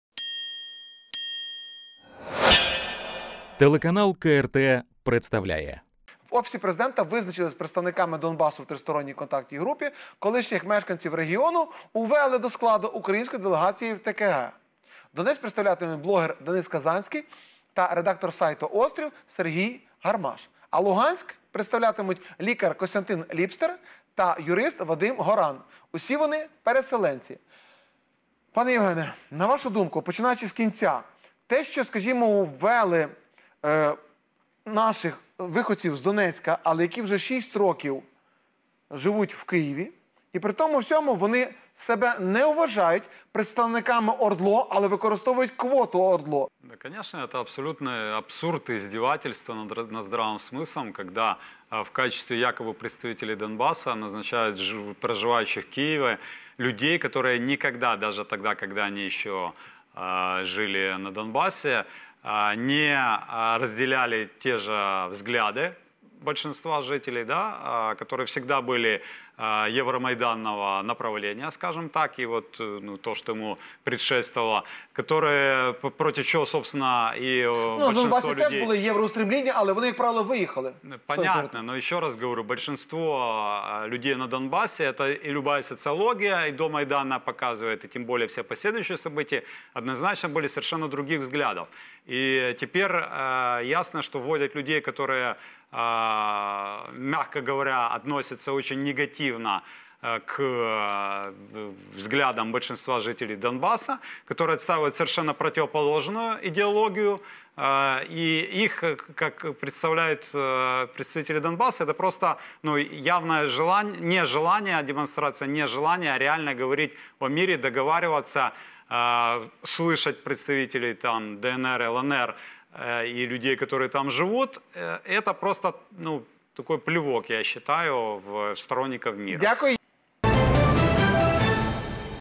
Зеленский не желает говорить с Донбассом — Евгений Филиндаш // Прайм. Аналитика
Политолог Евгений Филиндаш считает, что выбор в трёхстороннюю контактную группу представителей Донбасса, которые уже 6 лет не живут там, говорит лишь о том, что президент не желает находить решение военного конфликта.